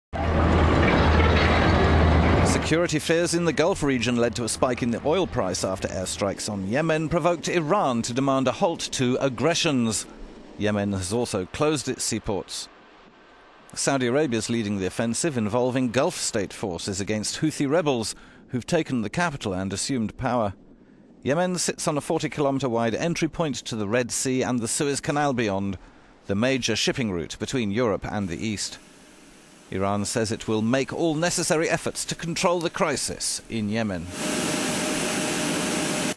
Air strikes in Yemen send nervous oil market up
Security fears in the Gulf region led to a spike in the oil price after air strikes on Yemen provoked Iran to demand a halt to "aggressions".
Yemen has also closed its seaports.

Saudi Arabia is leading the offensive involving Gulf state forces against Houthi rebels who have taken the capital and assumed power.
Yemen sits on a 40-kilometre wide entry point to the Red Sea and the Suez canal beyond, the major shipping route between Europe and the East.